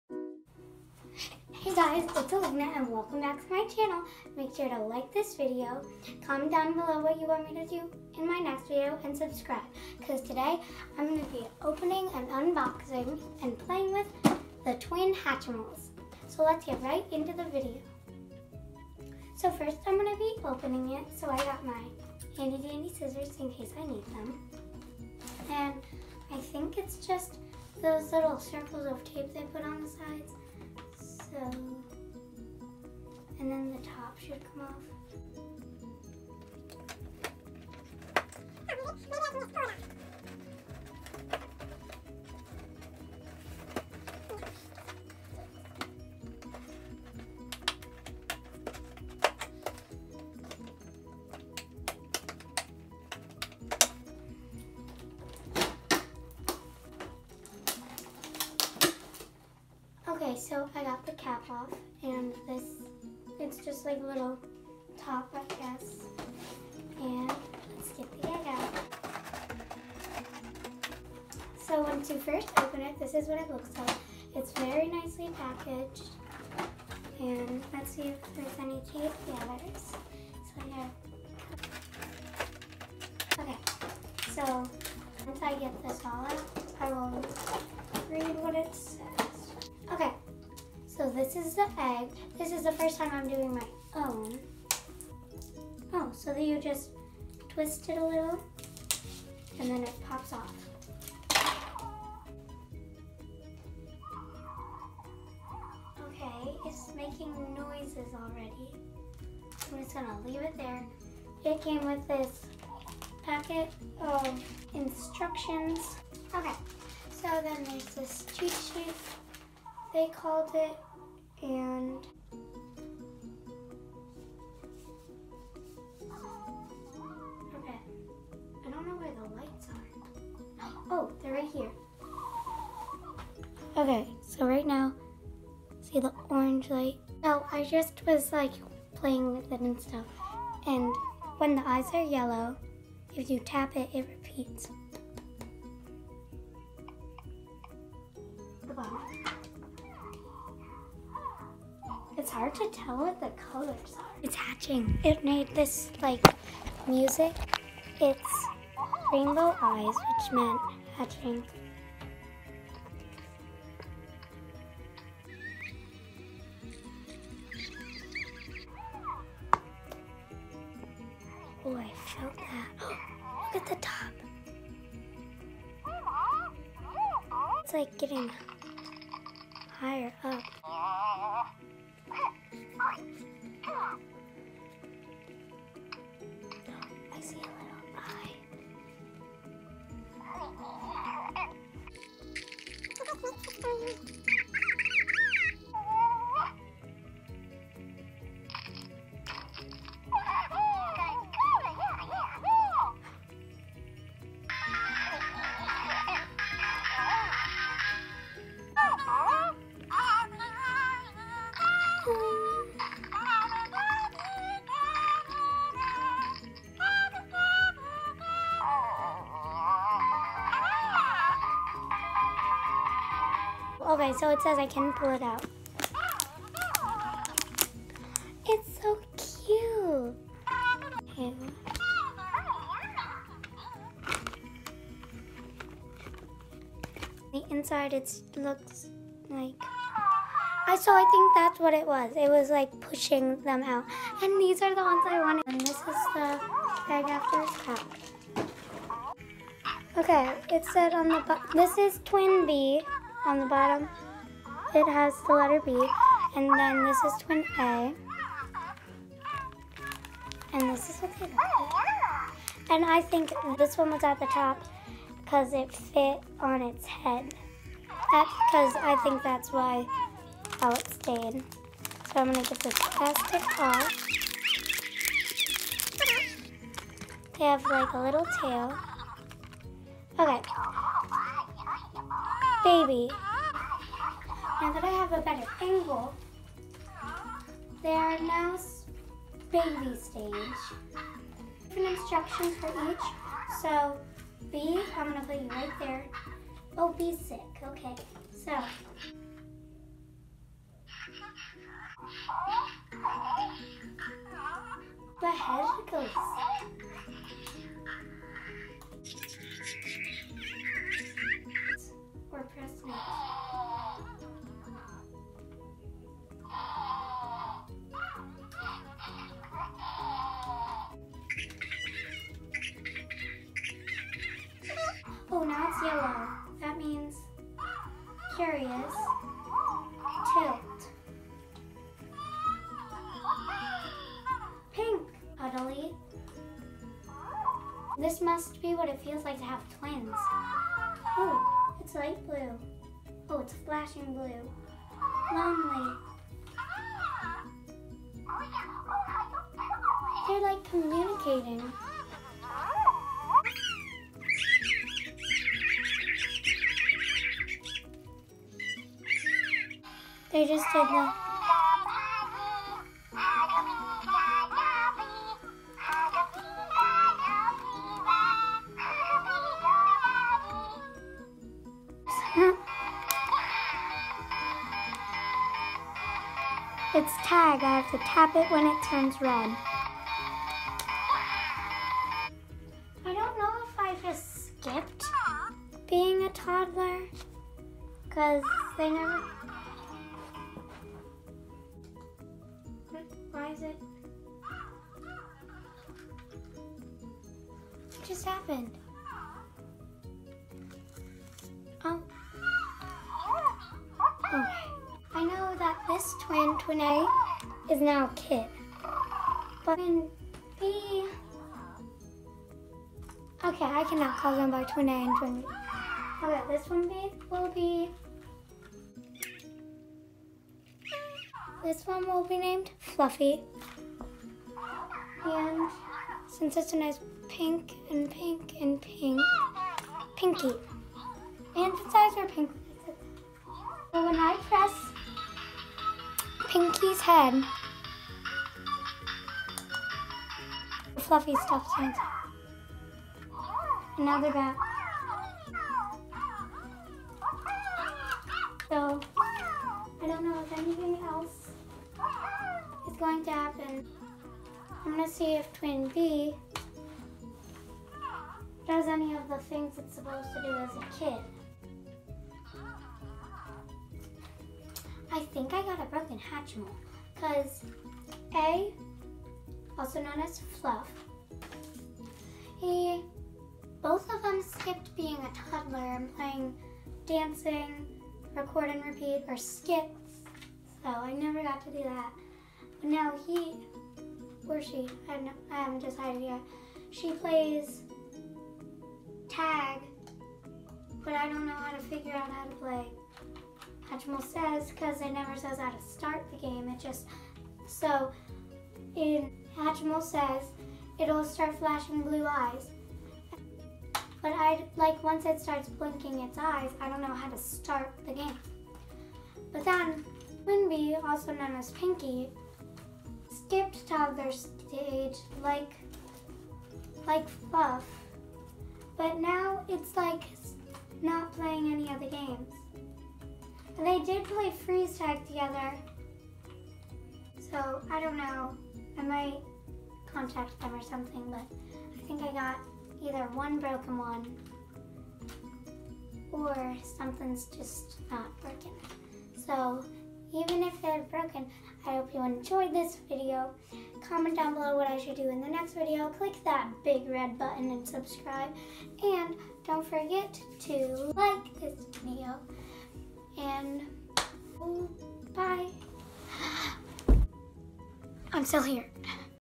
Broken Hatchimals?!?!
The Hatchimals I got with my own money didn’t work!!

~Elena😁💖